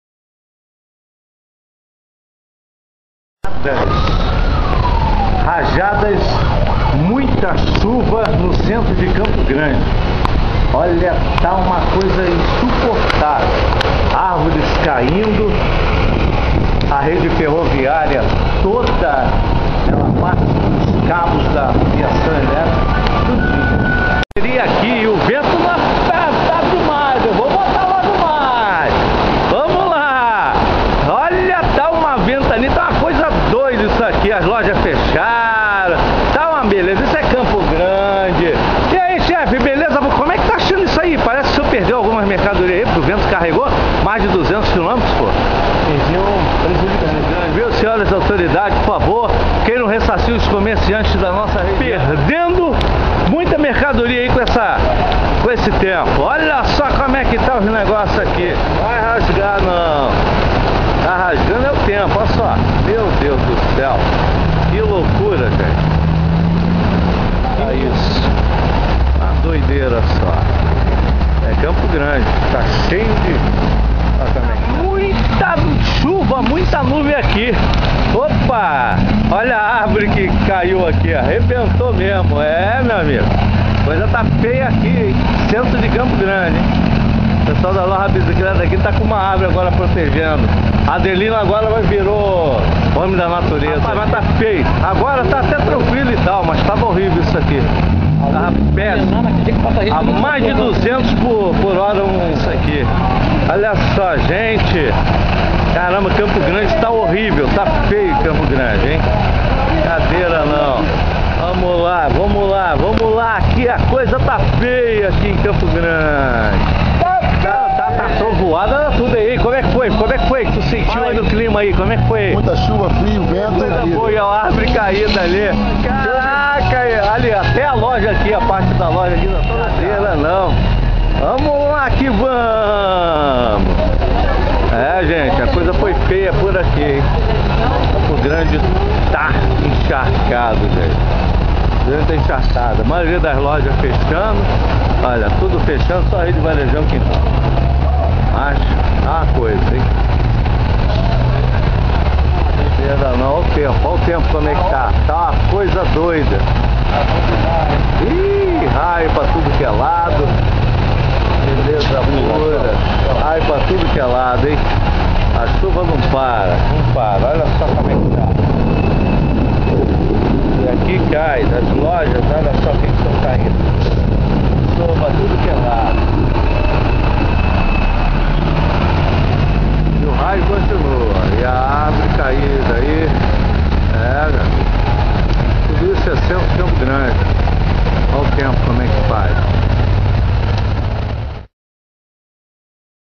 CHUVAS ARRAZAM O CENTRO DE CAMPO GRANDE DO RIO_xvid.avi
Vários trechos entre as Ruas Ferreira Borges e Aurélio de Fiqueredo no bairro de Campo Grande, RJ, Zona Oeste e da Região Metropolitana estiveram sem energias por exatamente 1 hora e 10 minutos, fazendo com que vários comércios fechassem suas portas, com receio de tumultos e por alguns minutos continuavam sem luz. A Light informou que os locais sem o serviço são Campo Grande a bairros circunvizinhos, como também Santa Cruz, Mendanha, Rio da Prata, Guaratiba e Paciência.
 Em Campo Grande, uma arvore de aproximadamente 8 metros de altura, interrompeu a passagem dos pedestres em frente à Nova Bicicleta e Rede Varejão Hortifrutti na proximidade da Rodoviária de Campo Grande, e no sub bairro Rio da Prata, o estrago foi com as placas de publicidades próximas a Estrada do Cabuçu e algumas árvores que destruíram muros de algumas casas.
Segunda a empresa, o problema aconteceu devido à chuva que atingiu a fiação da Linha Férrea da Estação de Trem de Campo Grande, que partiu ao meio com um raio e em várias partes da cidade que um dia foi chamado de maravilhosa.
A Light, o Corpo de Bombeiros e a Comlurb, disseram que devido ao forte temporão que suas equipes estariam atendendo conforme a demanda, informando que suas equipes estariam caminhando para os locais, trabalhando para restabelecer o fornecimento do serviço.
GRACIANO CASEIRO -- RADIALISTA, DIVULGADOR CULTURAL E ESCRITOR